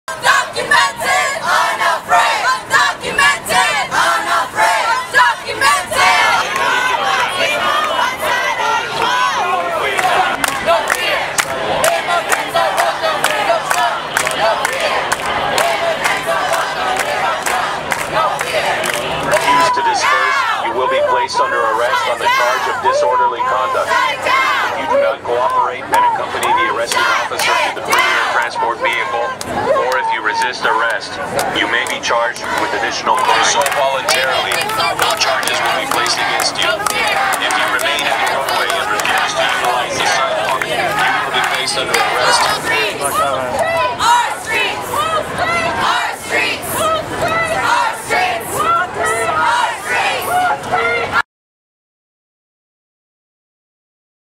Protesters busted at Trump Tower after President ends DACA
Protesters were arrested at Trump Tower in New York City after President Trump ends DACA.